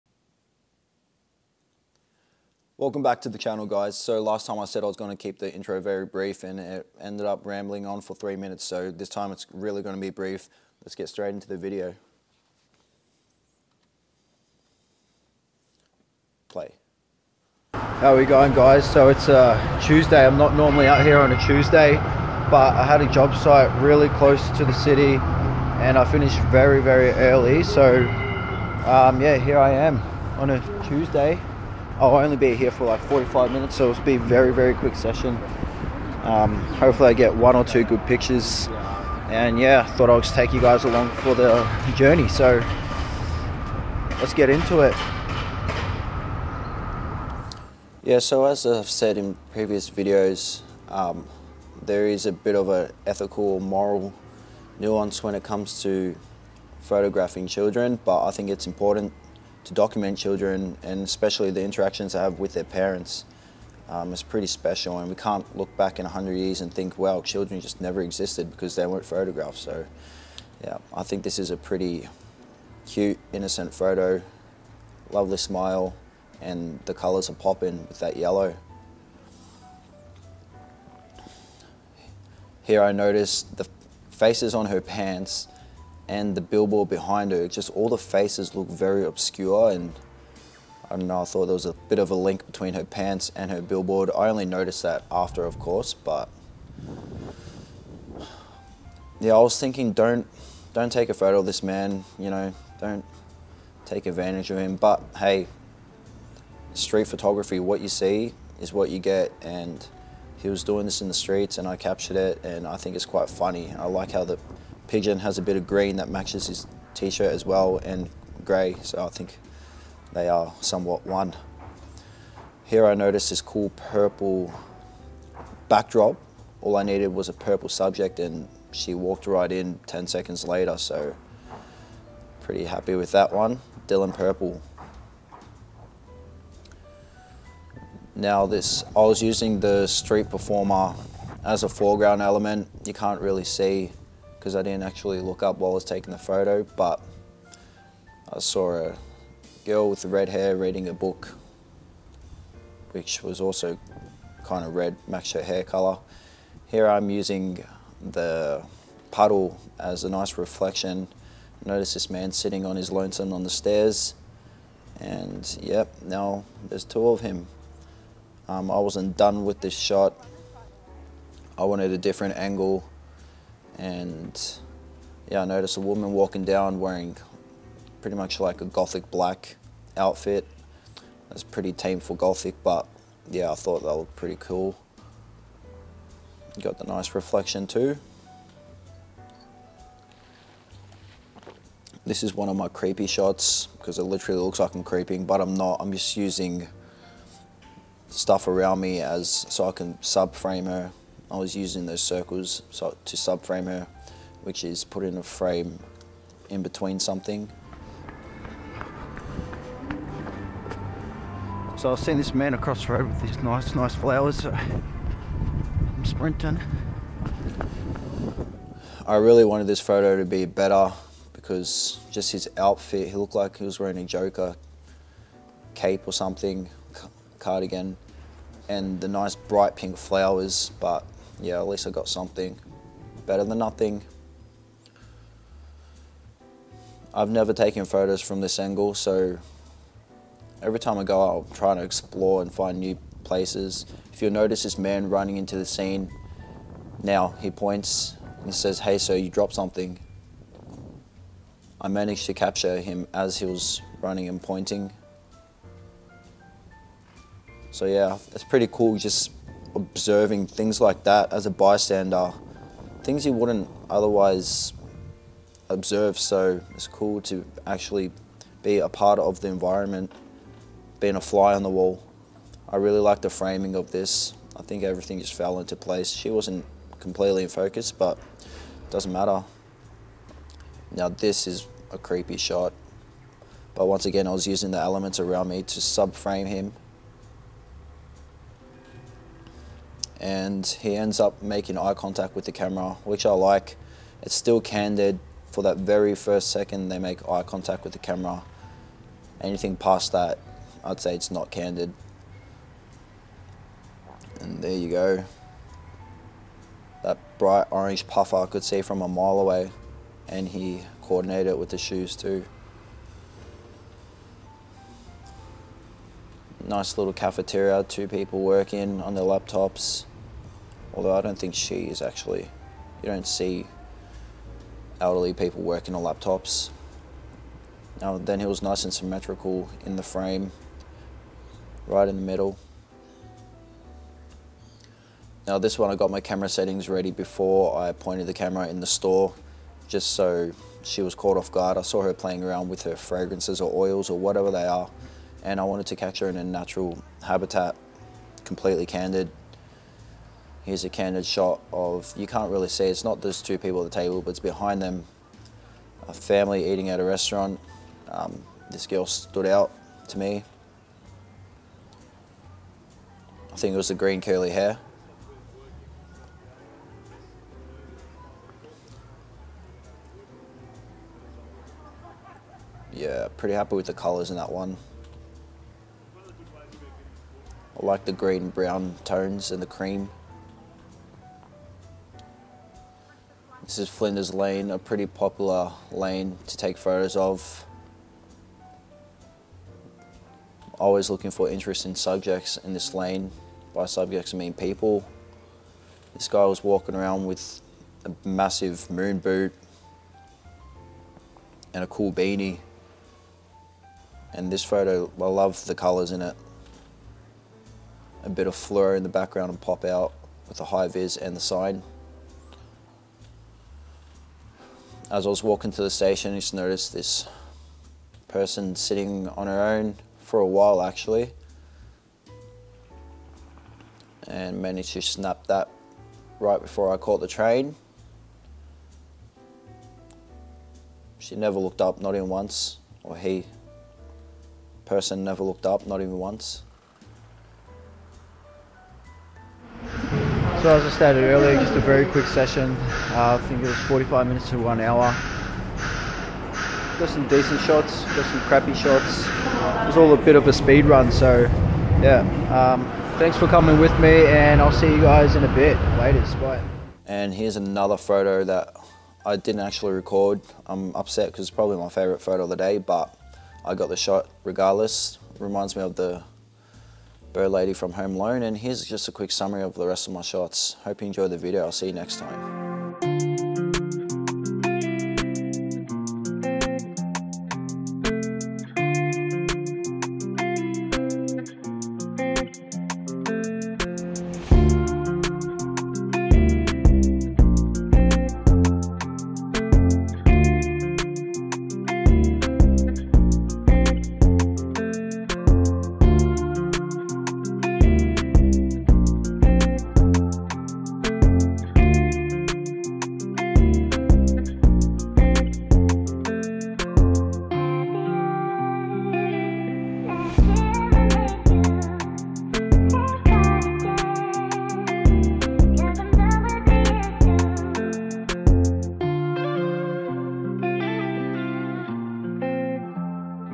Speed Run Street Photography with commentary EP 4
Welcome back to another POV street photography session where I only have 45 minutes on a quiet day to capture as much as I can.

My instagram is @player.characters